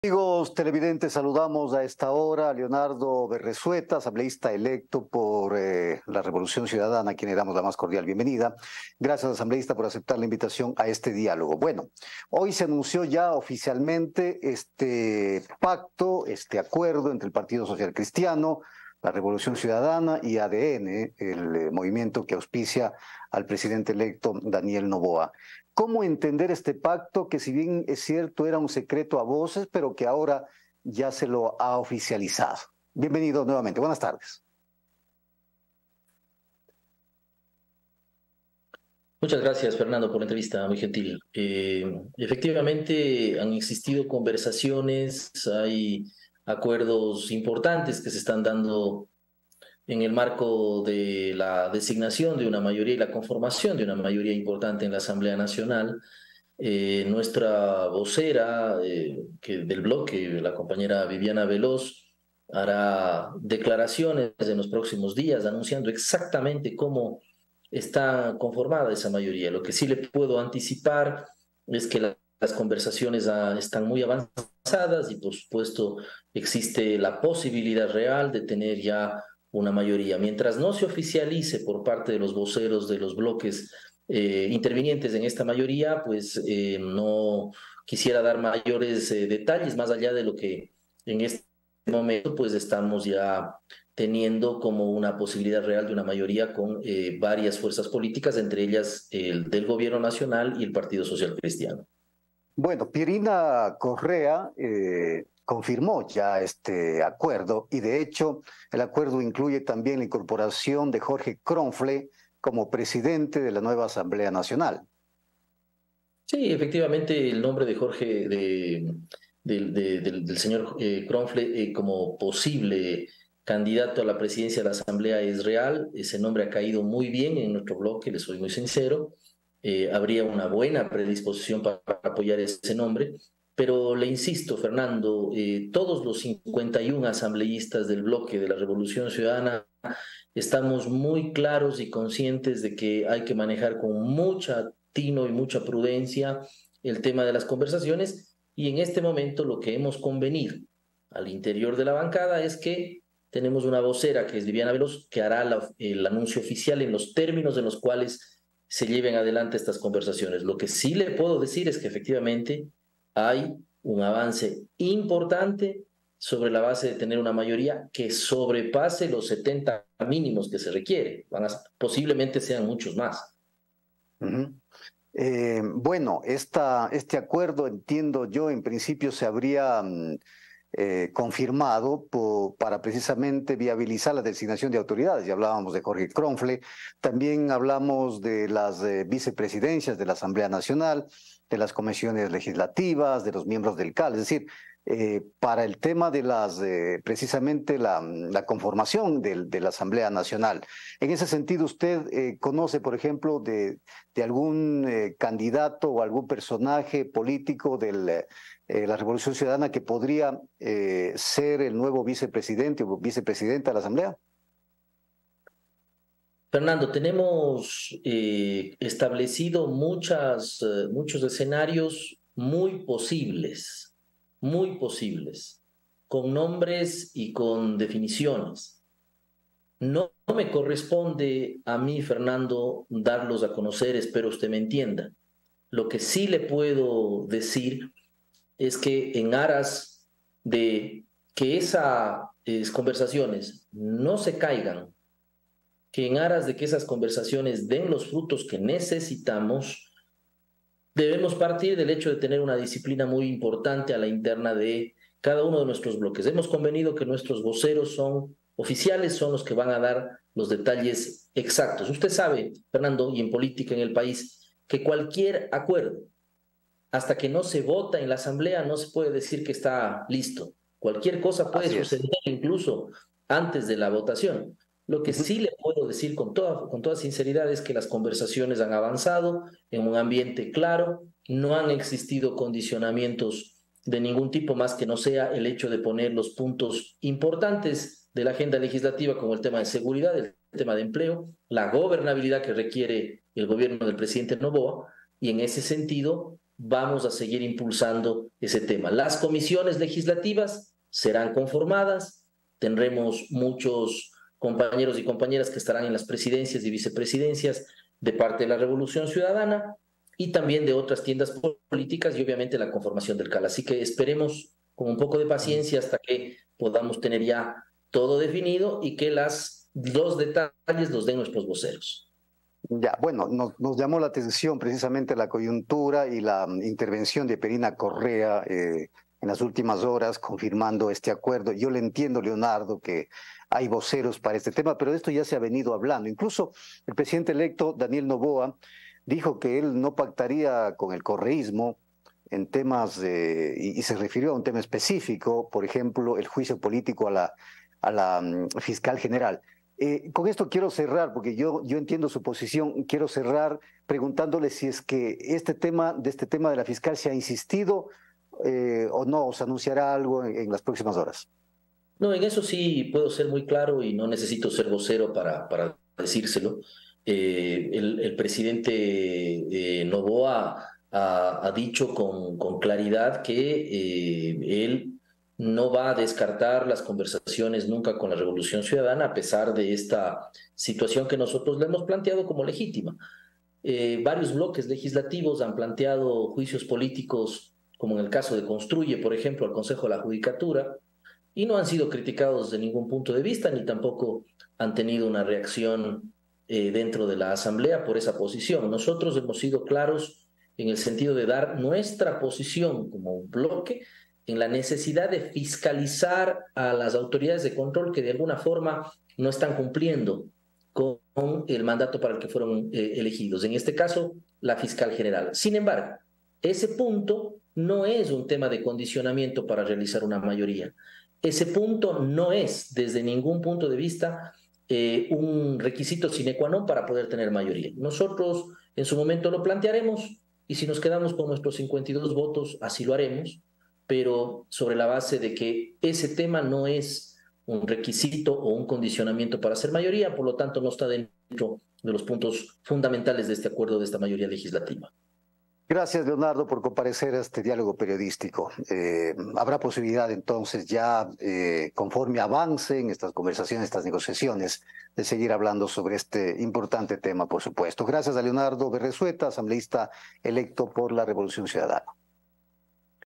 Leonardo Berrezueta
Asambleísta electo por Revolución Ciudadana
Tema: Conformación de la asamblea nacional